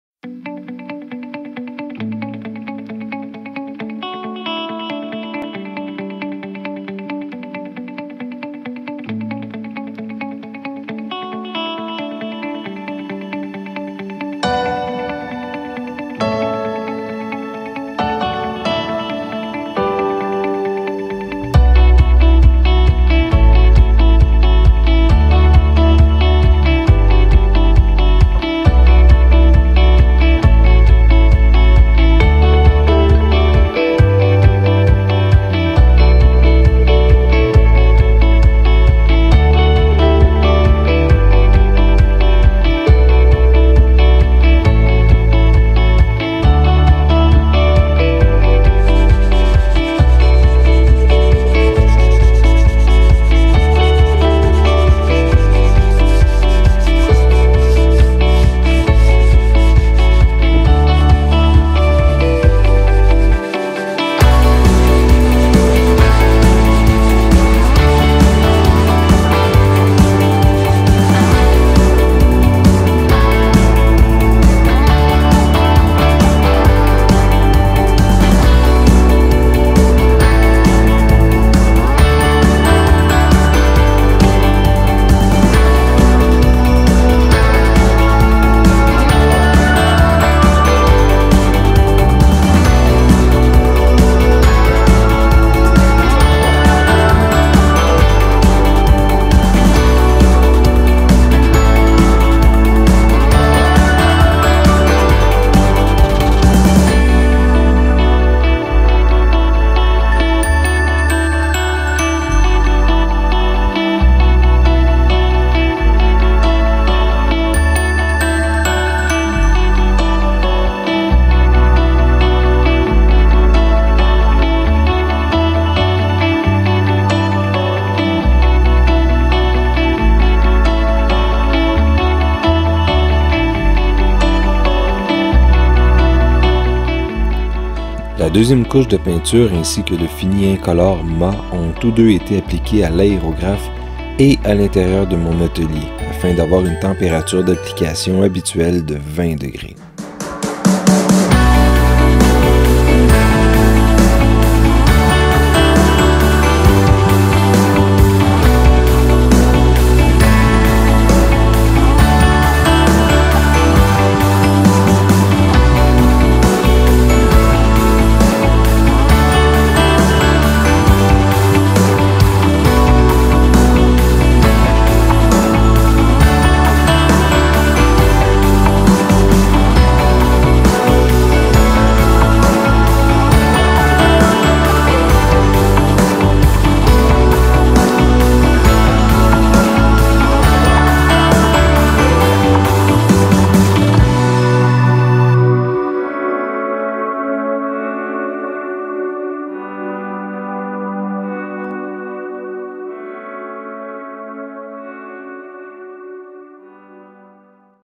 Fabrication d'un cadre de vélo artisanal en carbone - Ep. 6 - Peinture
Mise en peinture

La dernière couche fut appliquée à l'aérographe (buse 0.5mm) et à l'intérieur de l'atelier à 20 degrés. Malheureusement, les conditions ne me permettaient pas de filmer en même temps.